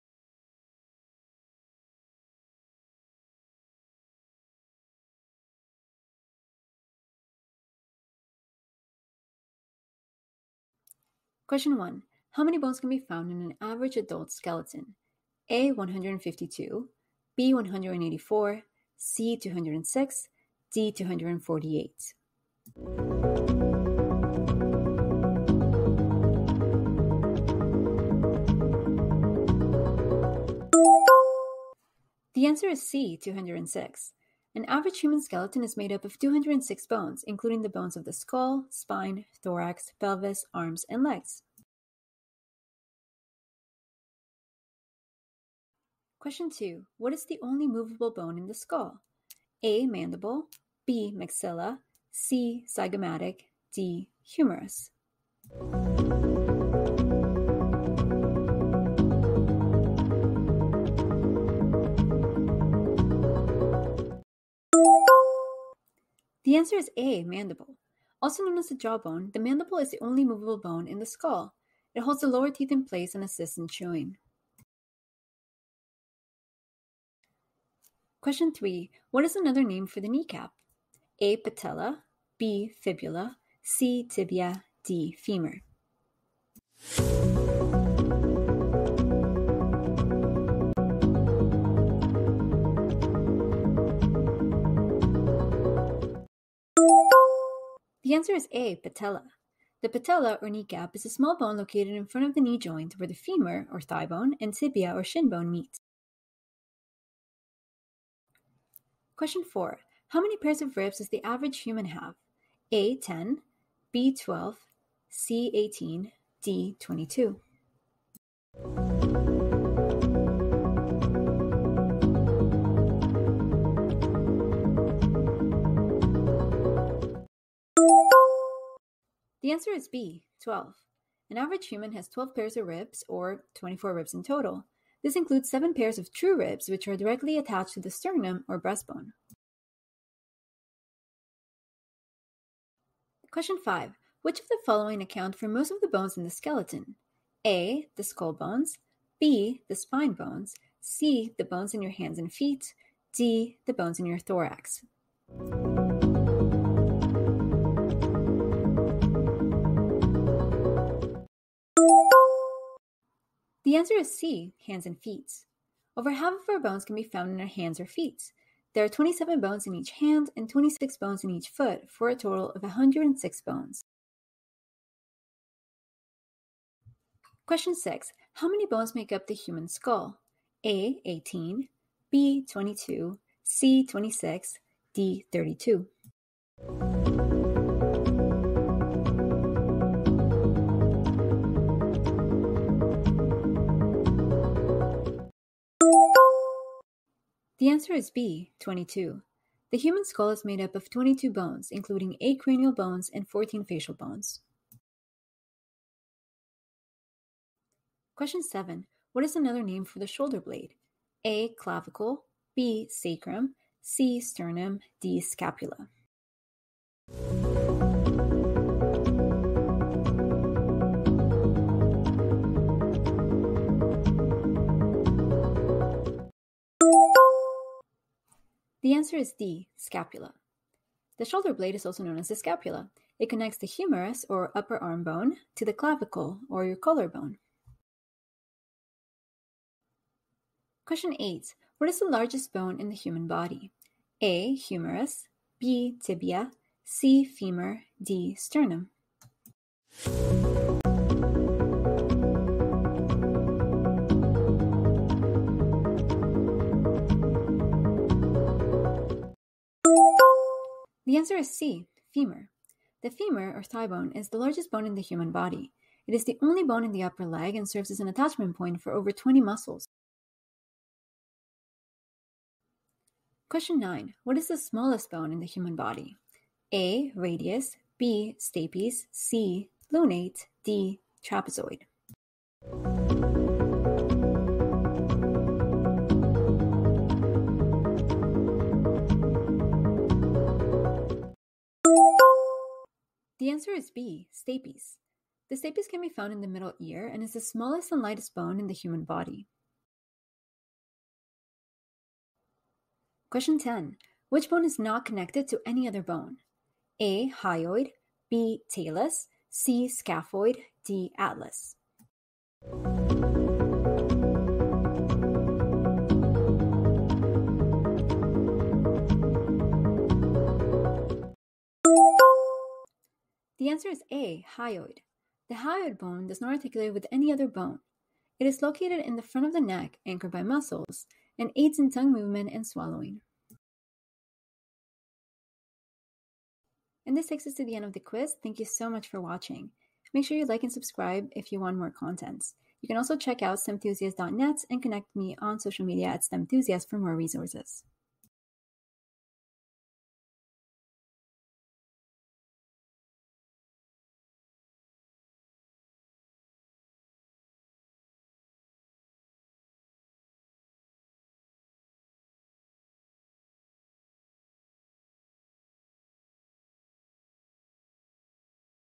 Anatomy and Physiology Quiz (Multiple Choice) | Skeletal System | STEM Education Trivia
New series: Anatomy Quizzes and Trivia✨

This quiz include 10 multiple choice questions about the skeletal system. Are you up for the challenge?

Drop a comment and let me know what topic you'd like to see me cover next.